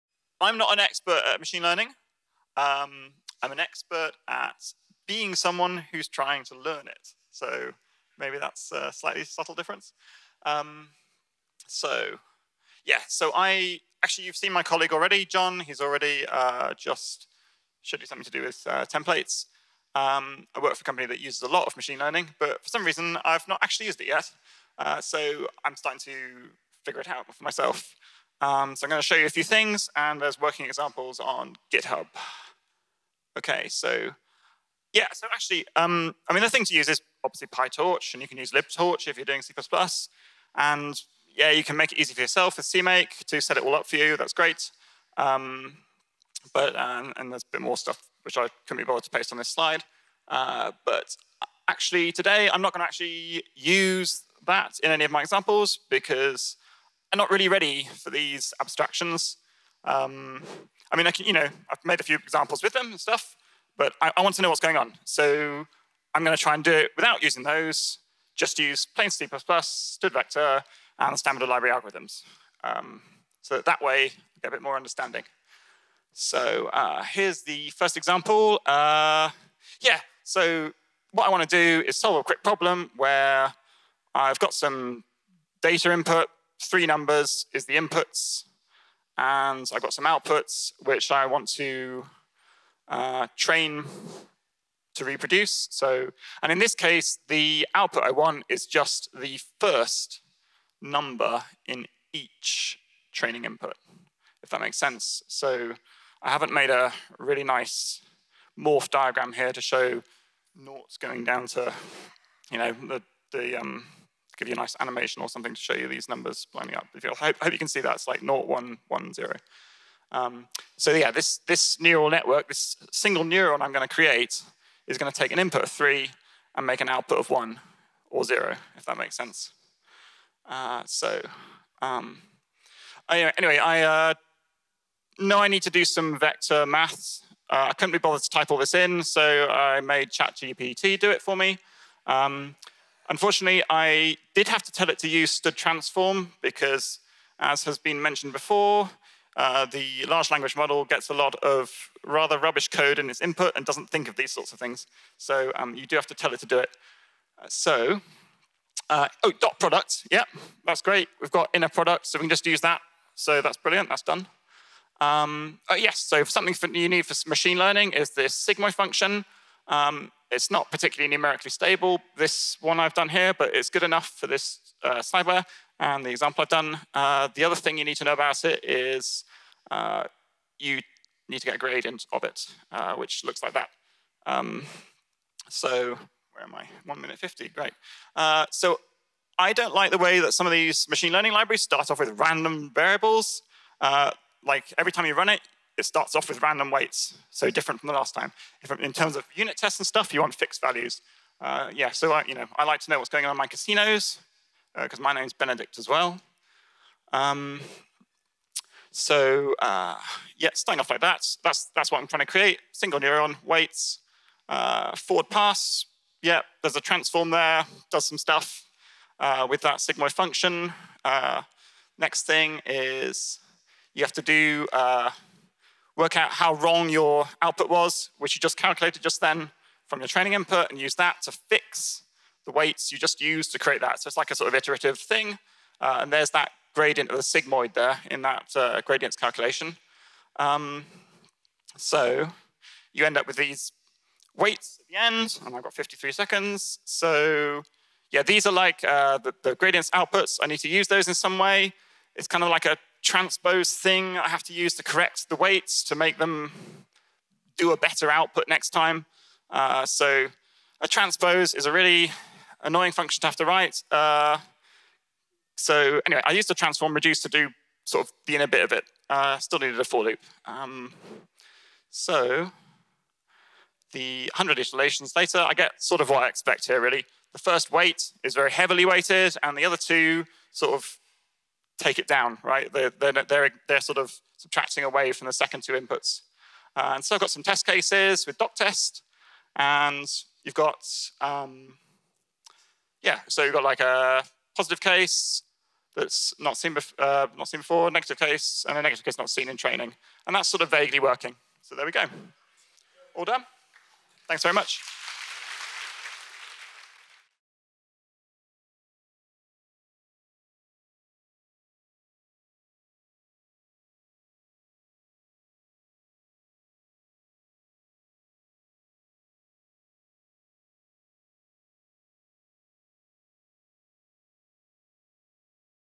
Lightning Talk: Really Simple Machine Learning in C++ - Ben Huckvale - C++ on Sea 2023
Join me on my learning machine learning journey, with a simple C++ example using std::vector and STL algorithms to train a single neuron.

Speaker

I write software for DNA sequencing devices, but have also been in the financial sector and particle physics. I like demystifying stuff.

C++ on Sea is an annual C++ and coding conference, in Folkestone, in the UK.